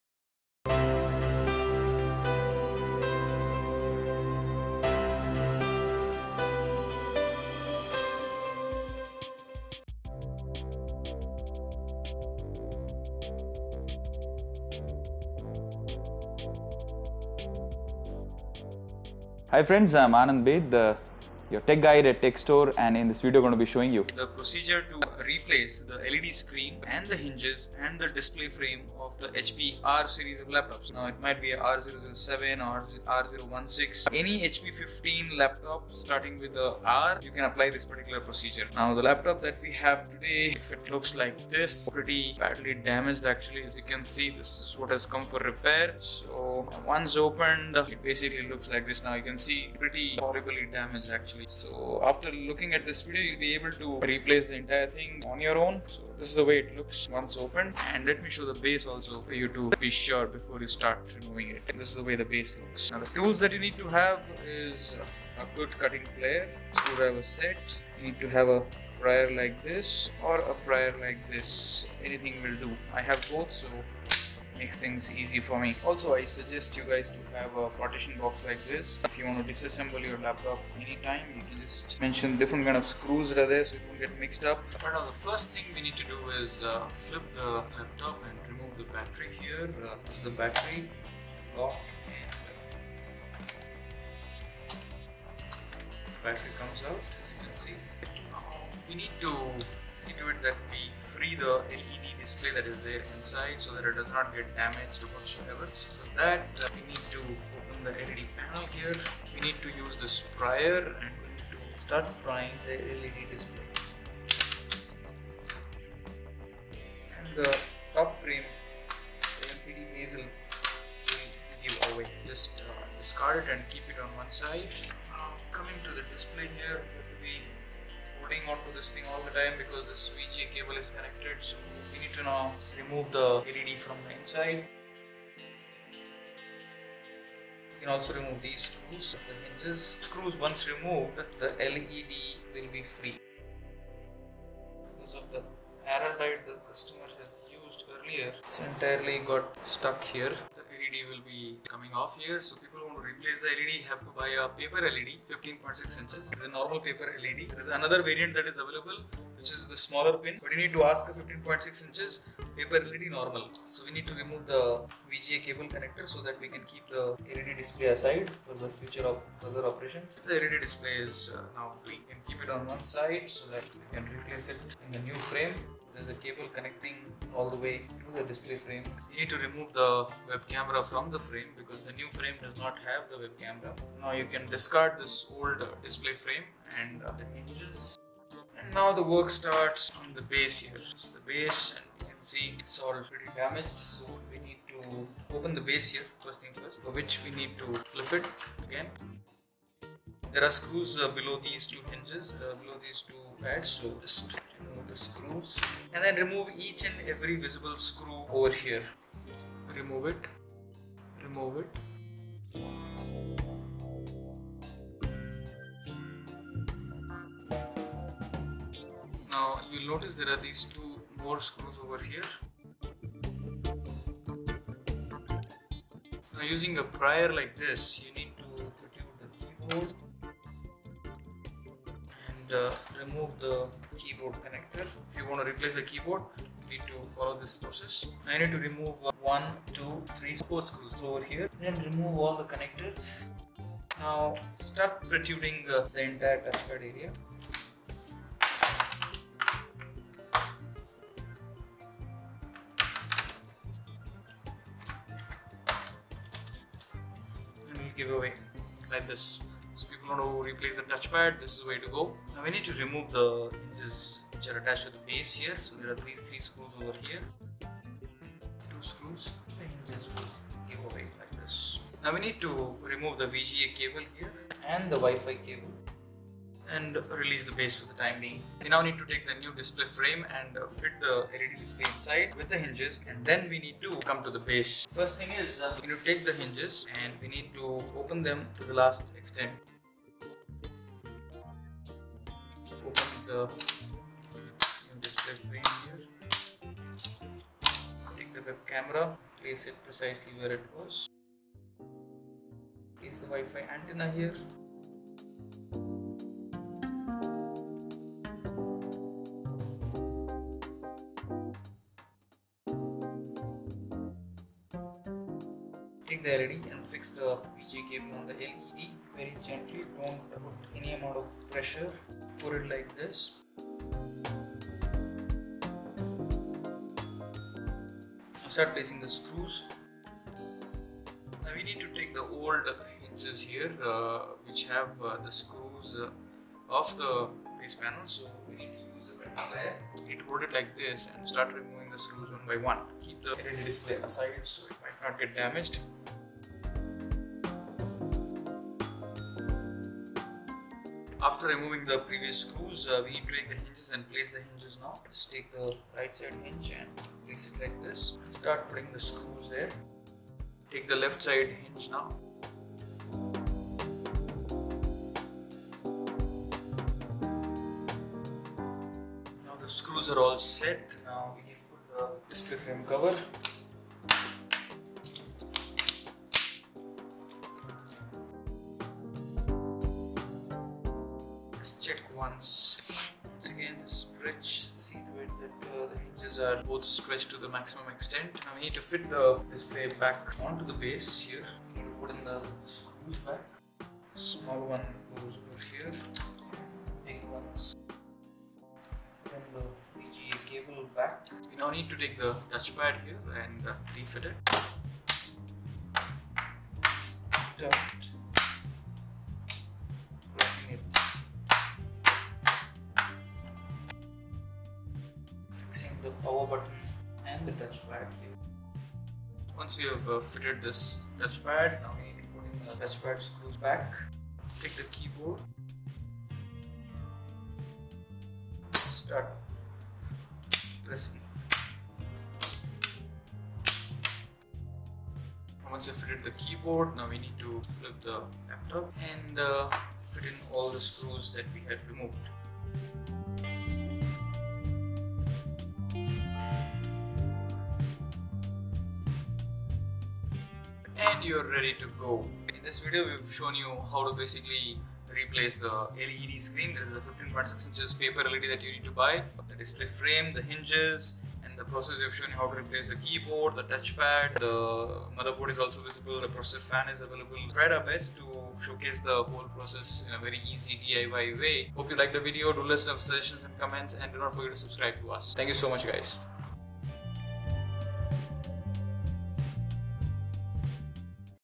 HP 14R 15R Series Hinges display frame: How to disassemble the base touchpad motherboard easy diy
- Watch this video to replace the led display frame hinges keyboard base motherboard of the HP 15R 14R series laptops. This laptop has a design flaw due to which most laptops have this issue. After watching the video, you will not face the same problem.